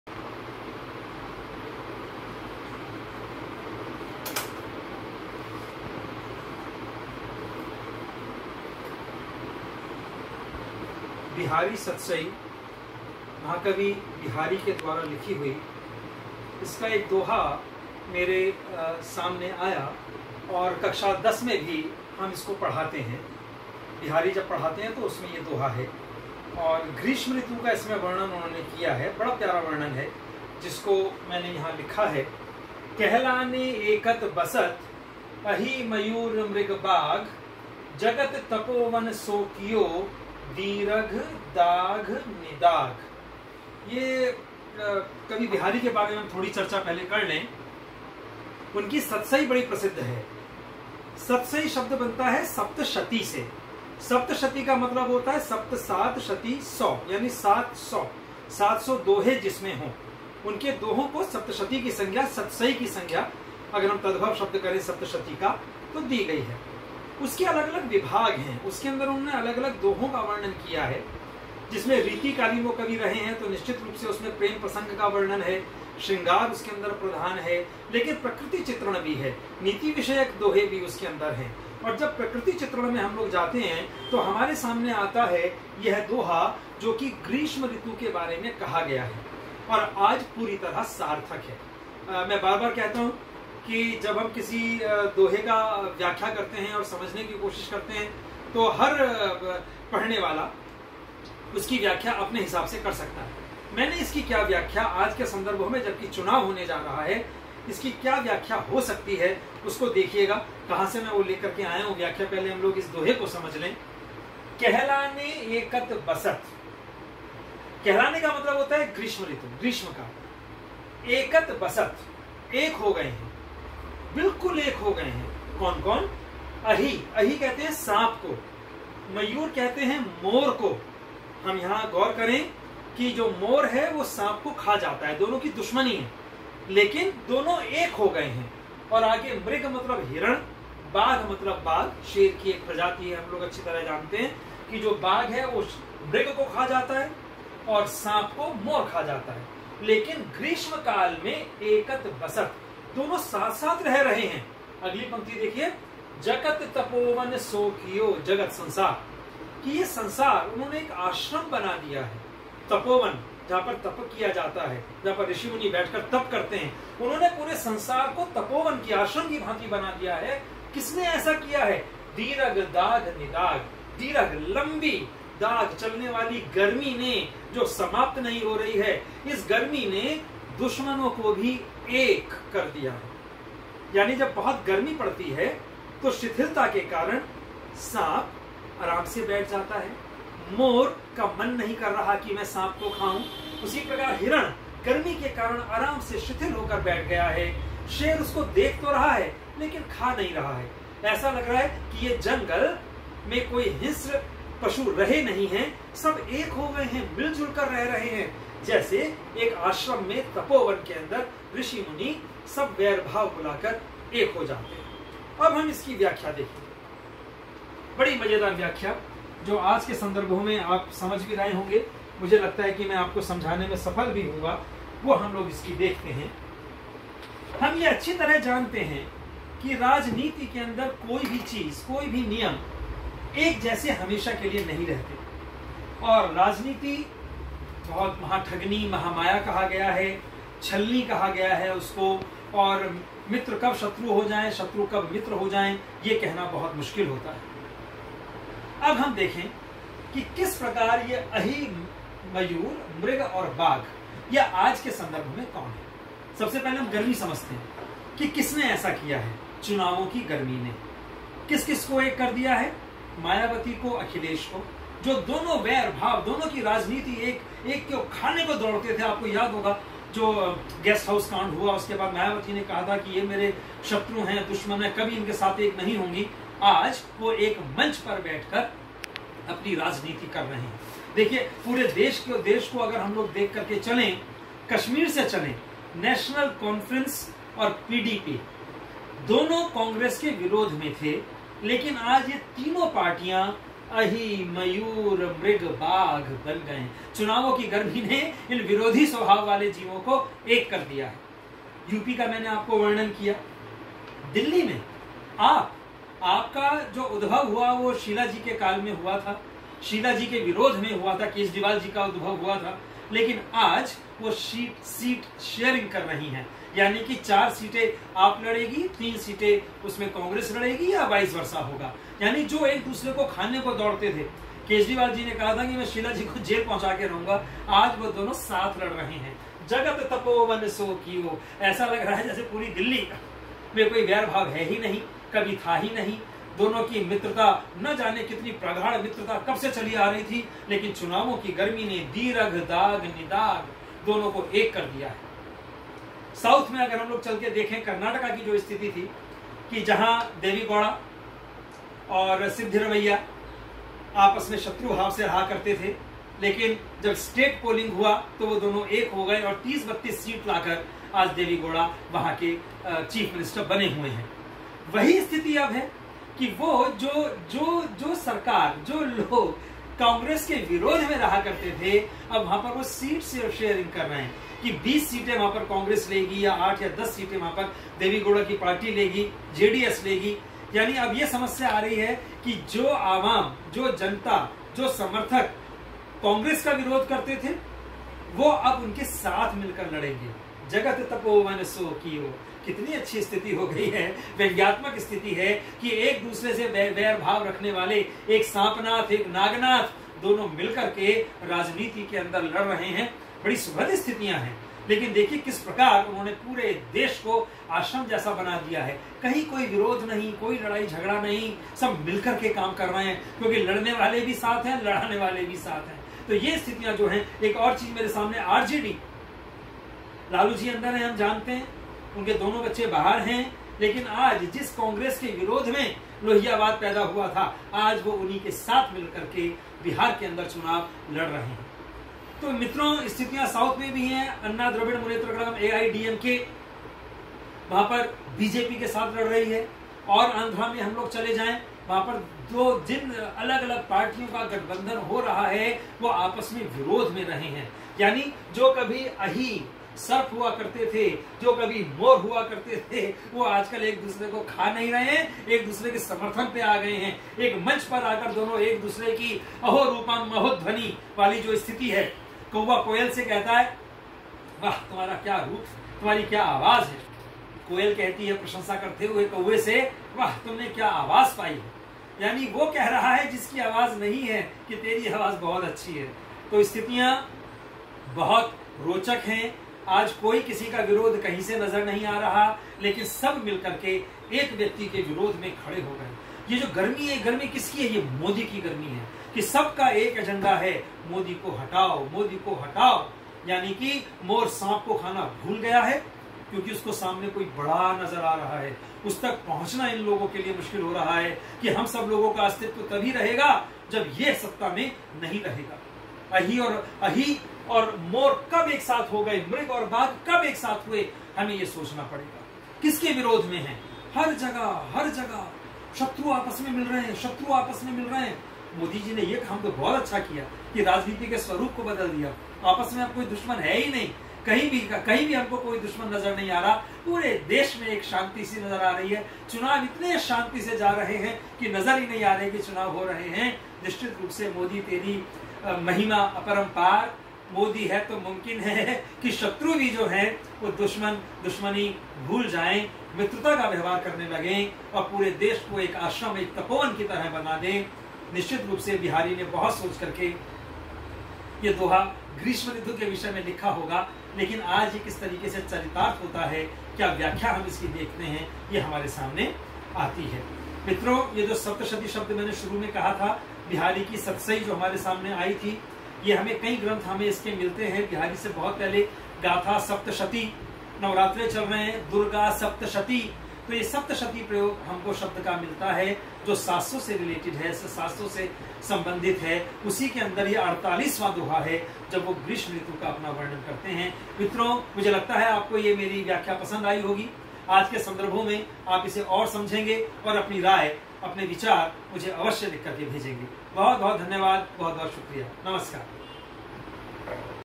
सम्यक दृष्टि-6,डॉ प्रदीप कुमार जैन,7-4-2019
कहलाने एकत बसत, अहि-मयूर, मृग-बाघ ।
जगत तपोवन सो कियो, दीरघ दाघ निदाघ ।। बिहारी
आज के माहौल में यह दोहा पूरी तरह सार्थक है।चुनावों की बढ़ती गरमी ने किस तरह देश को तपोवन बना डाला है ,ज़रा सुनिए और लोगो से भी share कीजिये।अपनी राय अवश्य दें।